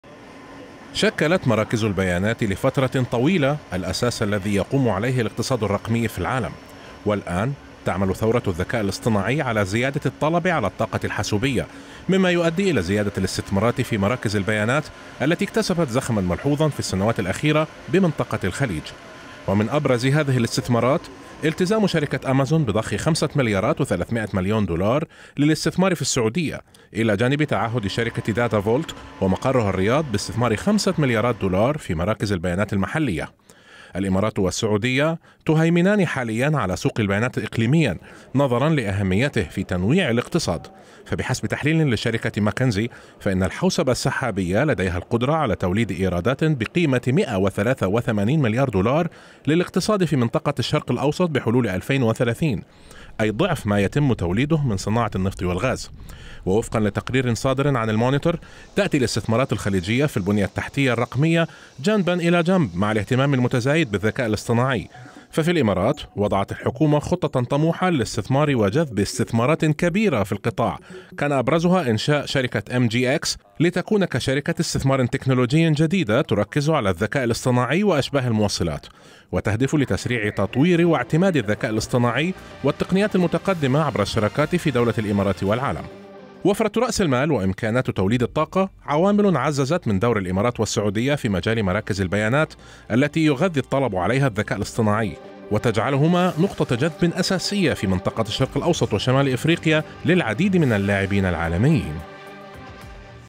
دول الخليج.. استثمارات مليارية في مراكز البيانات
مع ازدياد الاهتمام بالذكاء الاصطناعي في منطقة الخليج، برز الاهتمام أيضا بمراكز البيانات والاستثمار فيها، فلماذا اكتسب هذا القطاع هذه الأهمية؟ ولماذا تركز دول الخليج على جلب هذه المراكز بدلا من الاعتماد على الحلول المقدمة من الشركات العالمية في الخارج؟